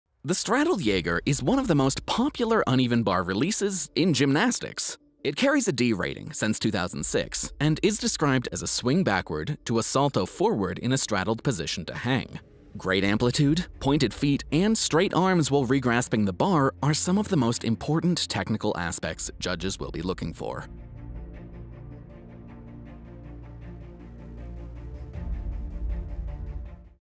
amplitude ✔️
The Straddled Jaeger is one of the most popular uneven bar releases in gymnastics.

D-Score
Fails
Falls
Sticks
Spotters
Sporteverywhere
Gymnastics
Simone Biles
2020
Tokyo 2021